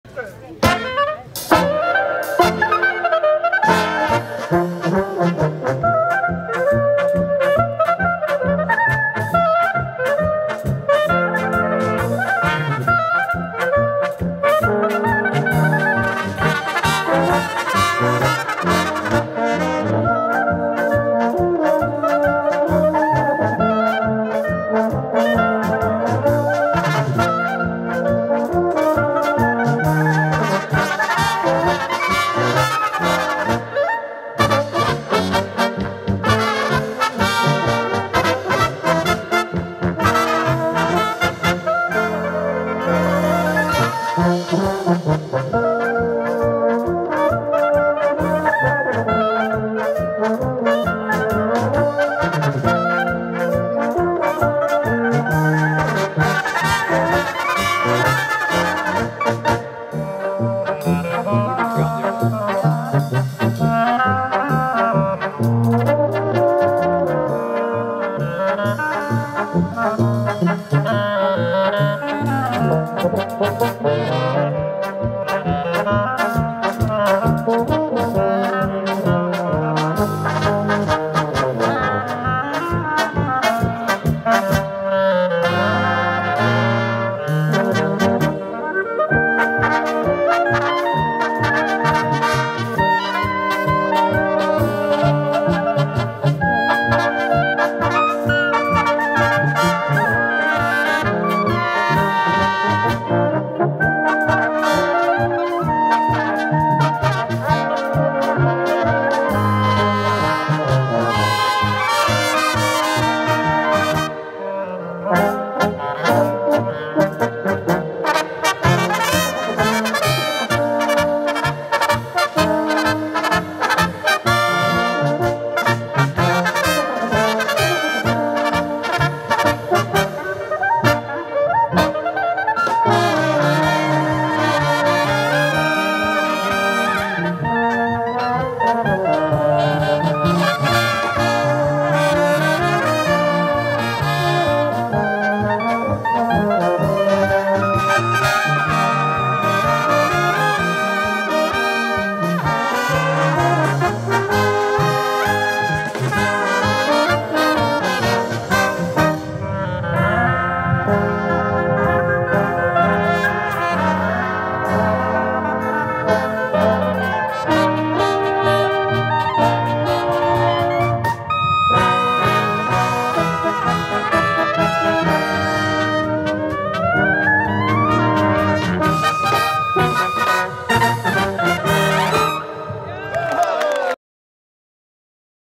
Freunde Echo - klarinet solo
Pieter van der Loo speelt een klarinet solo met Freunde Echo uit Overloon tijdens het Frühschoppen bij 't Helder in Overloon.
Wie de titel van het nummer kent mag het zeggen in het commentaar onder de video